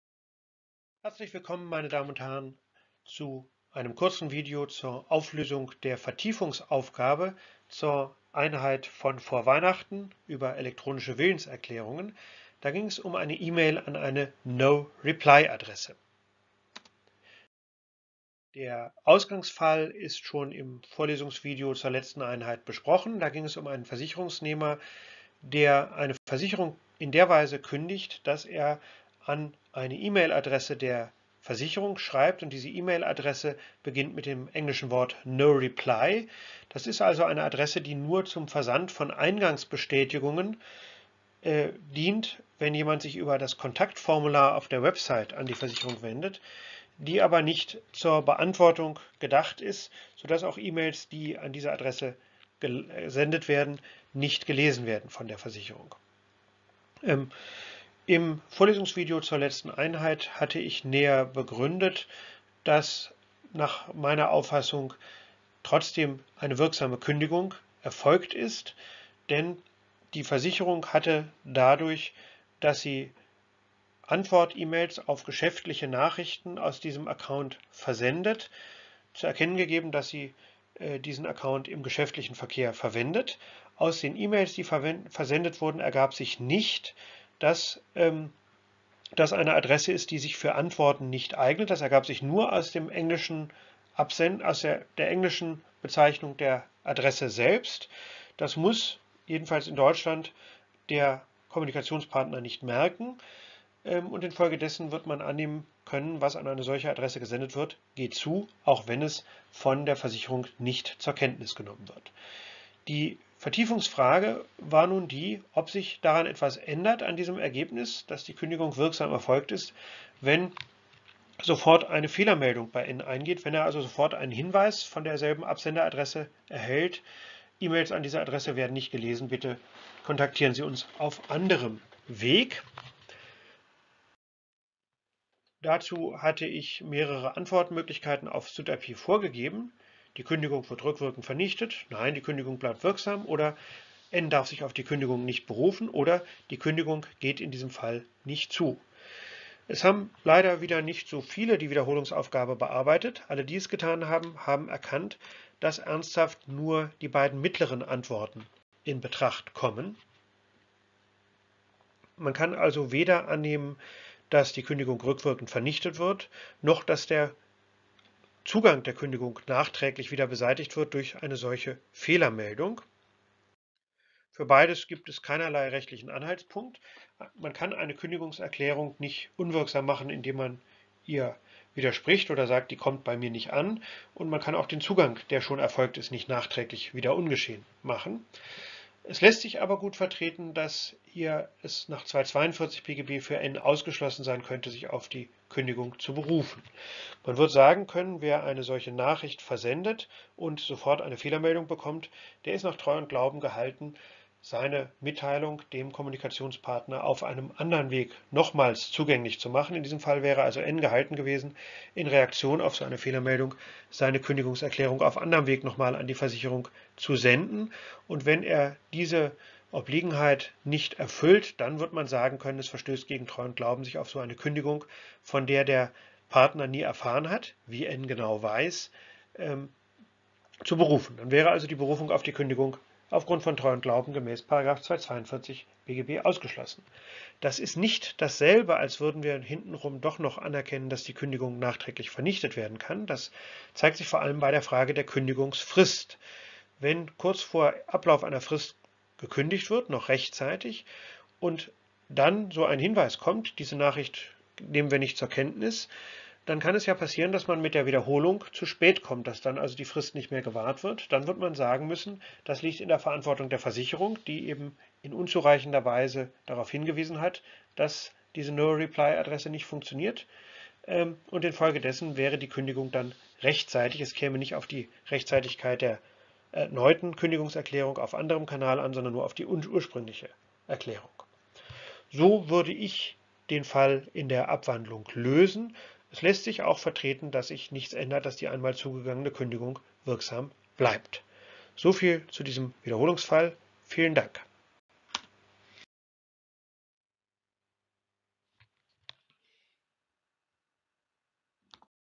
E-Mail an Noreply-Adresse
Auflösung zur Vertiefungsaufgabe der vorigen Einheit. Verwendet ein Unternehmen im Geschäftsverkehr eine noreply-Mailadresse ohne hinreichend darauf hinzuweisen, dass Antworten an diese Adresse nicht gelesen werden, so geht eine an diese Adresse gesendete Erklärung gleichwohl zu, auch wenn sie ungelesen bleibt. Was aber gilt, wenn der Absender sofort eine Fehlermeldung erhält, die ihn darüber informiert, dass seine Nachricht nicht gelesen wird? Bleibt dann z.B. eine Kündigungserklärung gleichwohl wirksam?